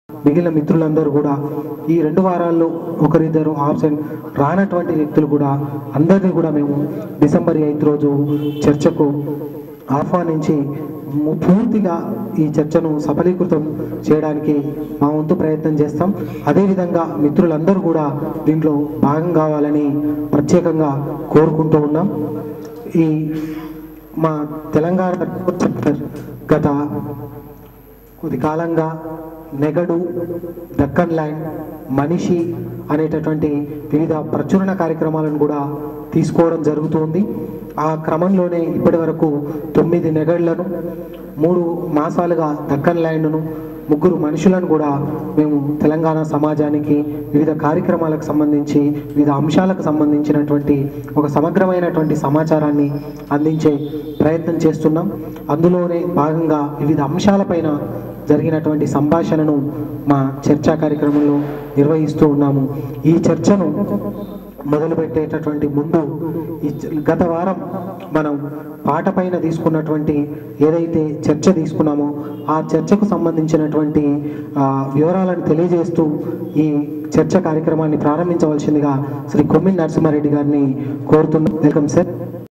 Somesh Charch-46
I N V I T A T I O N
PART-II
*TELANGANA RASHTRA SADHANA -- PAATA PAATHRA*
Saturday' 01st December, 2012 at 4.30 p.m
SPEAKERS
Kavulu, Gayakulu:
Sri ANDESRI
Sri RASAMAYI BALAKRISHNA
Sri ARUNODAYA RAMA RAO
Sri DARUVU ELLANNA
Sri PILLAM SANTHOSH
Sri V.M. YELLAIAH
Sri BAIRAGI MOHAN
Sri DAYA NARSING RAO
Sri Warangal SRINU. B
Sri JAKKA GOPAL
Sri NAGARAJU
Sri CHANDRA PRAKASH
Venue:
"CHANDRAM" 490, St.No.11, Himayatnagar, Hyderabad
Vedakumar.M
Chairman, Telangana Resource Centre (TRC)
Note (Web Casting) TRC-CHARCHA-46-Part-2 @ 5.15 p.m. onwards
Kindly ensure to be present by 4.15 p.m. to have "TEA" with Friends